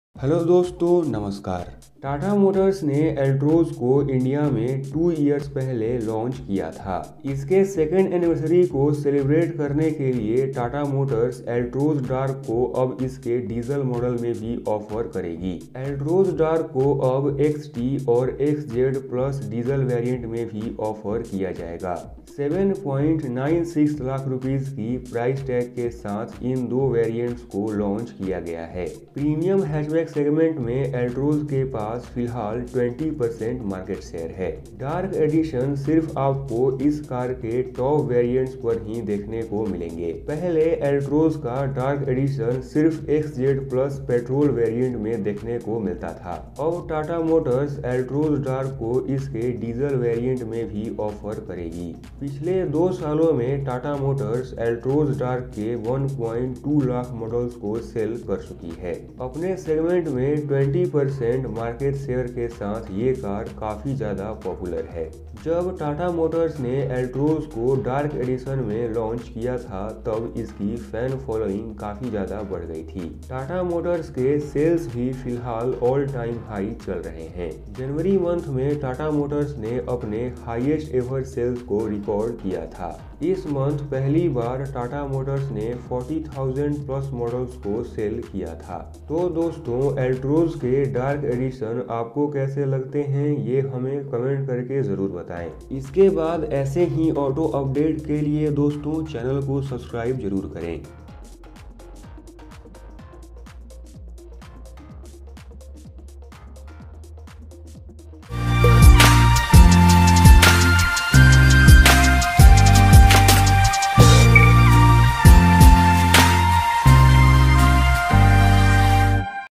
Tata Altroz Dark launched in XT & XZ+ Diesel variant | Price Rs 7.96 Lakh
Tata Motors is celebrating the second anniversary of the Altroz with the introduction of two new variants of the Altroz Dark edition

The Altroz Dark edition is now available in XT and XZ+ (diesel) trims, with prices starting at Rs 7.96 lakh (ex-showroom, Delhi)